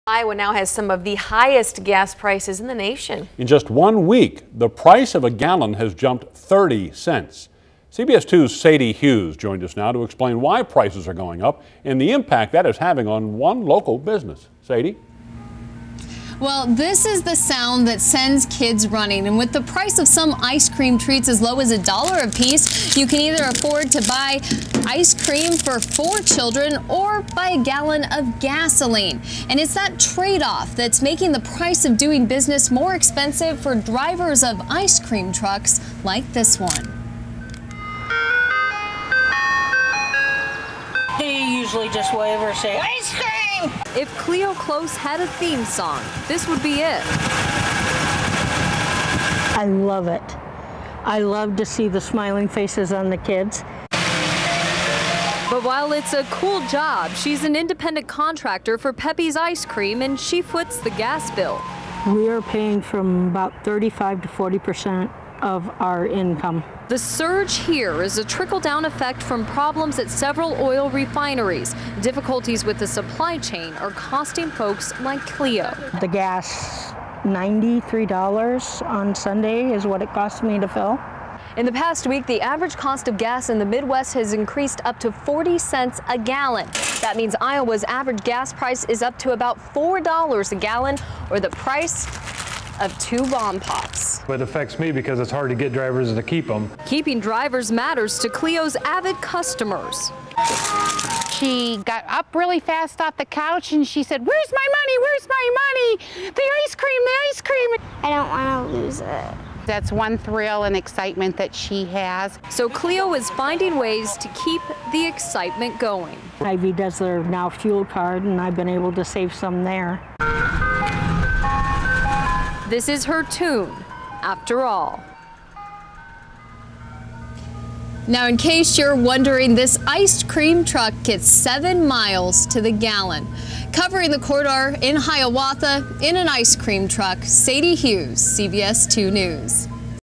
Ice Cream & Gas Prices
My photographer, Scott Zimdar and I were assigned a dreaded "gas prices" story. So, we decided to put our heads together and get creative, this is what we came up with.